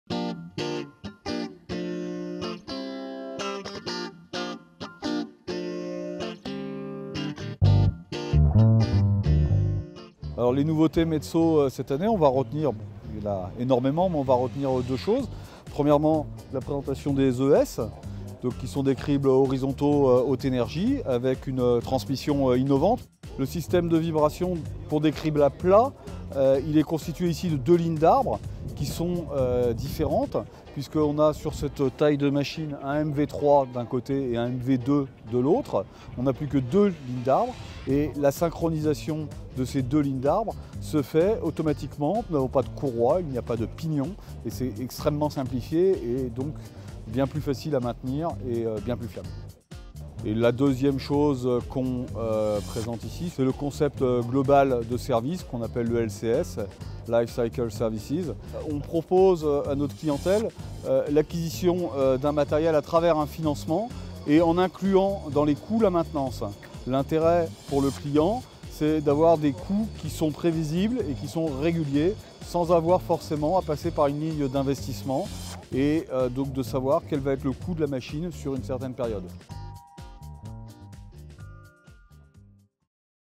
Metso de retour sur la Bauma
Après plus de 10 ans d’absence, le spécialiste finlandais des installations de carrières Metso Minerals signe son retour sur le salon Bauma. Une présence d’autant plus remarquée que son principal compétiteur suédois était absent. Présentation en vidéo du nouveau crible Metso Premier ES303 et de l'offre de services Metso LCS avec Grégoire Daviron, directeur général de Metso France.